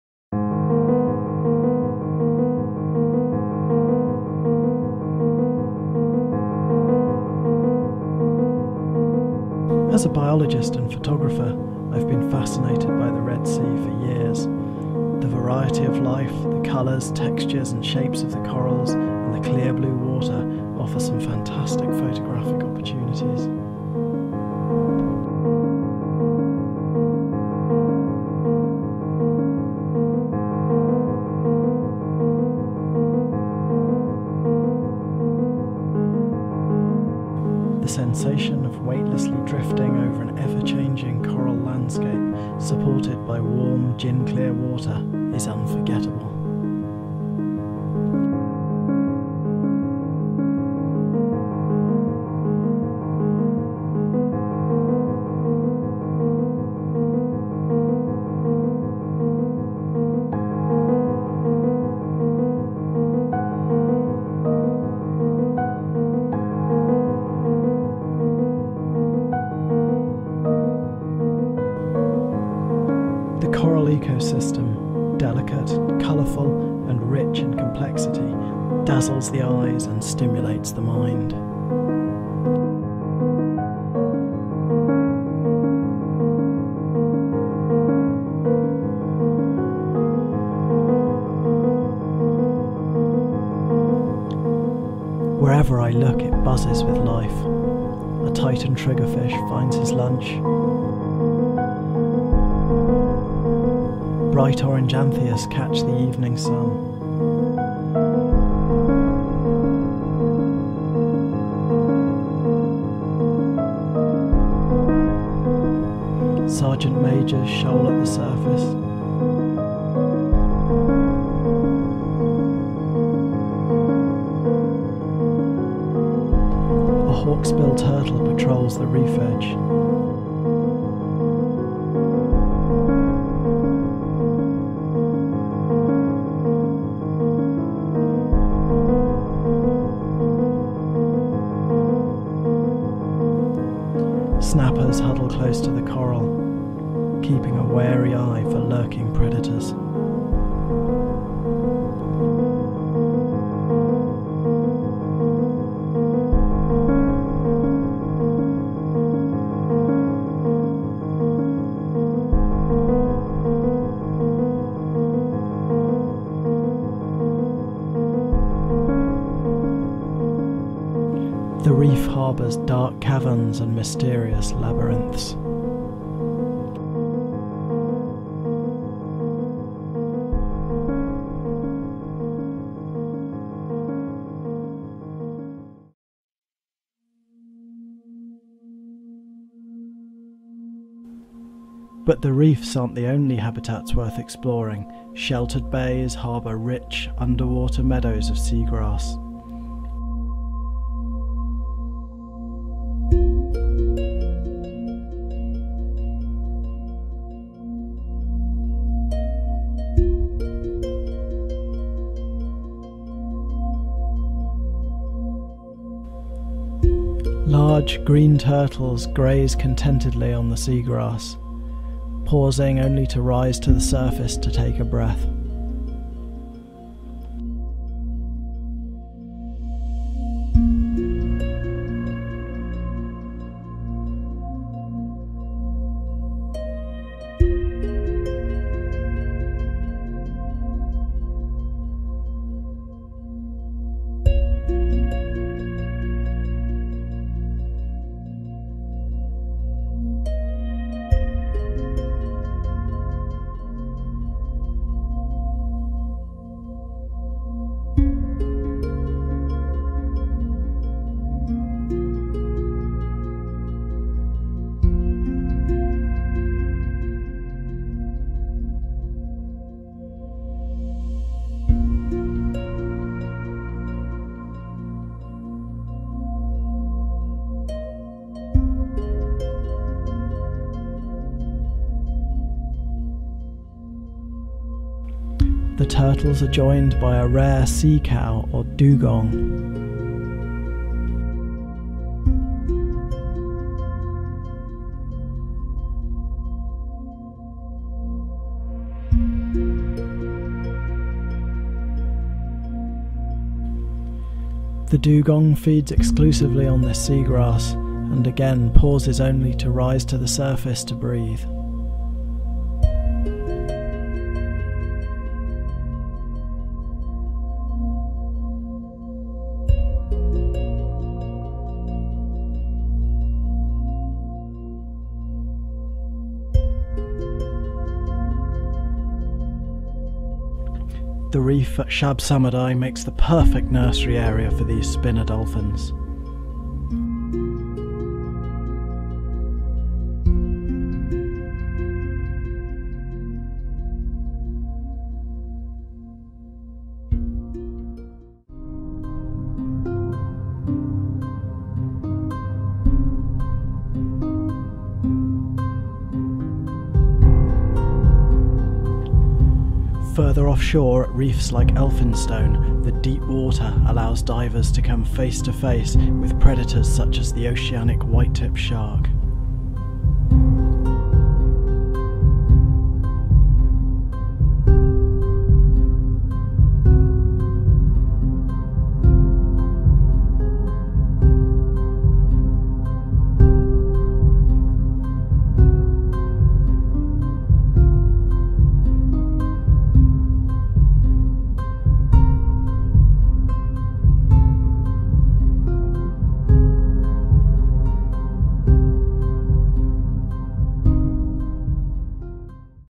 Under the Sea with Oceanic Sharks, Green Turtles, and Dancing Dolphins.
Ocean life under water